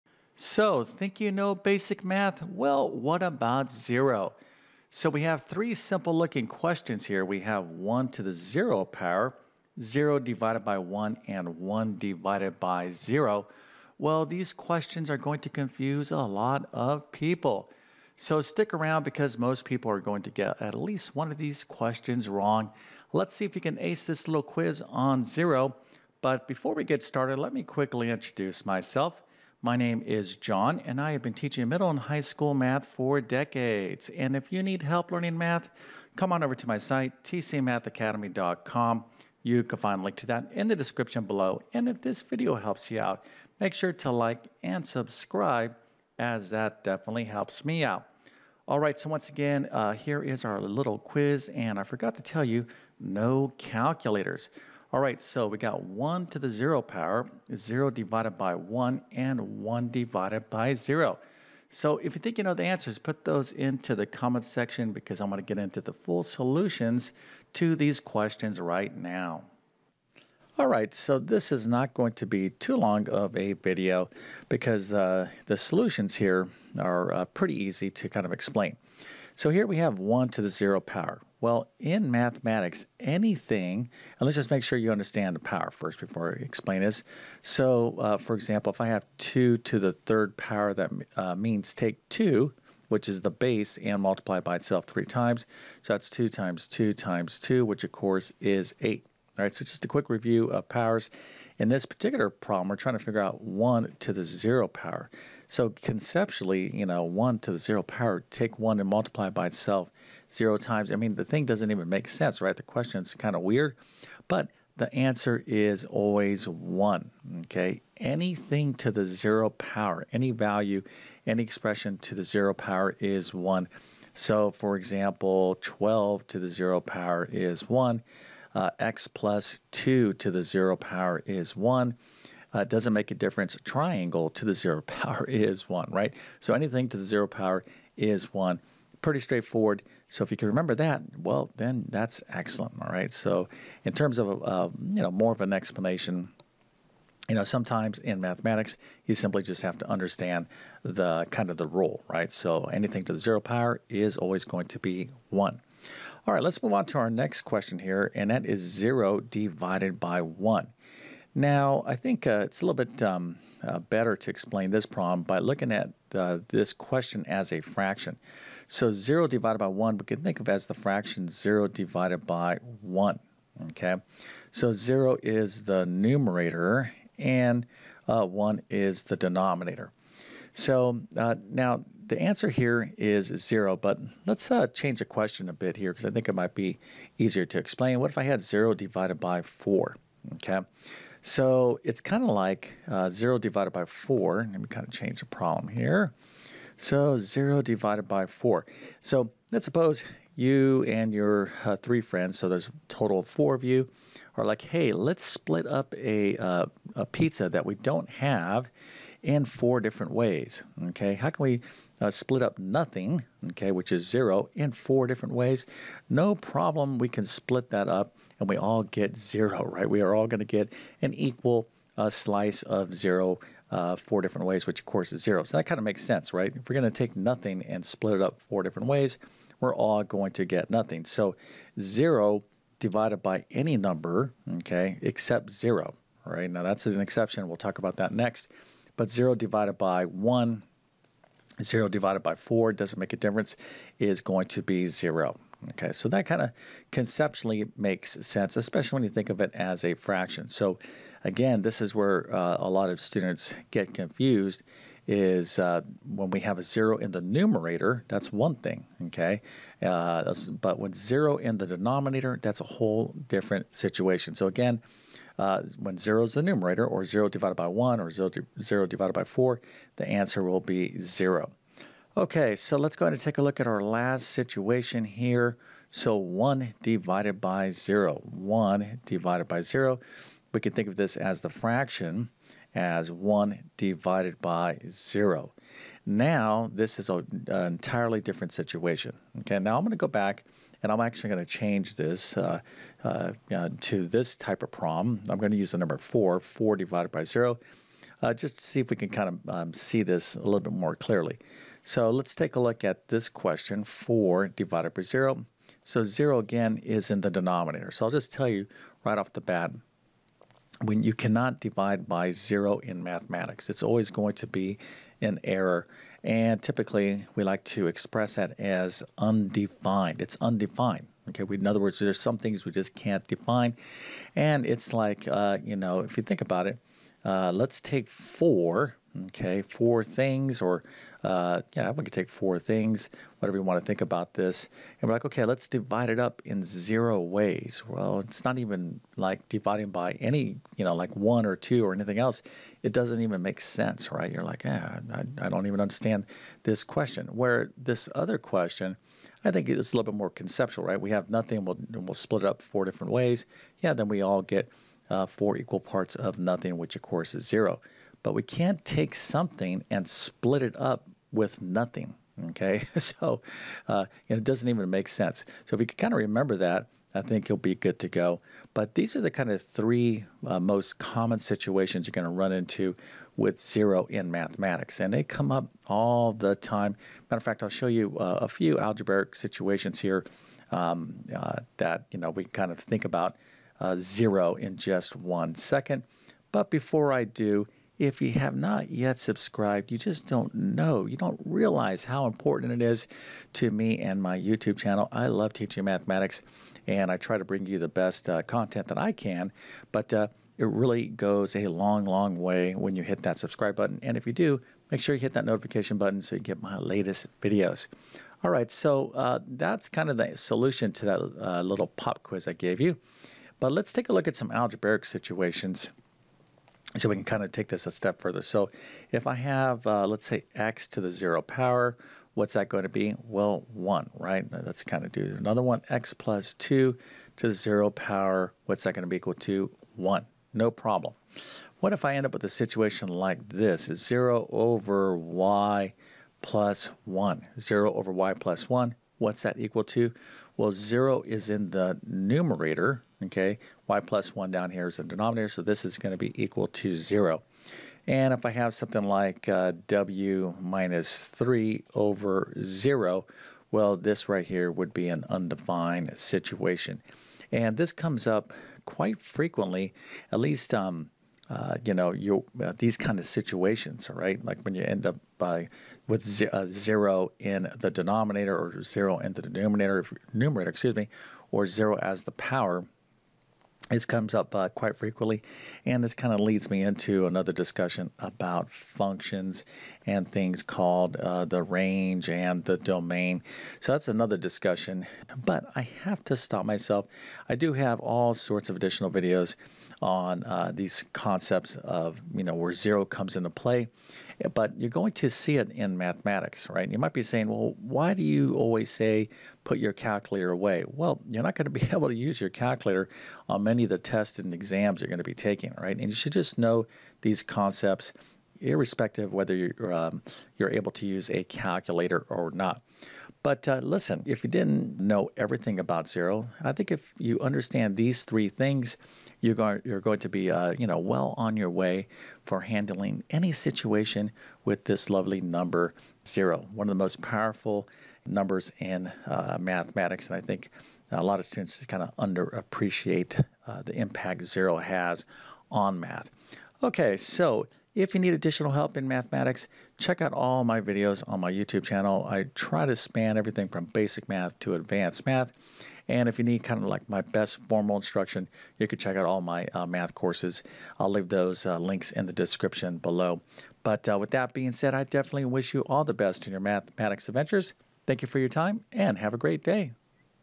1⁰, 0 ÷ 1, 1 ÷ 0 — 3 Zero Questions That Stump Everyone! 🤯 Can You Answer Them?
Can you solve 1 to the 0 power, 0 divided by 1, and 1 divided by 0? 🤔 You’d be surprised how many people get these wrong—even adults! In this video, I will break down the math behind zero step by step, so you’ll finally understand these tricky concepts!
📌 What You’ll Learn:
✔️ Why 1⁰ equals 1 (and why it sometimes confuses people!)
✔️ Why 0 divided by 1 is 0
✔️ Why 1 divided by 0 is undefined and what that really means
✔️ How understanding zero in math will make you sharper at problem-solving

• HOMESCHOOL MATH
• COLLEGE MATH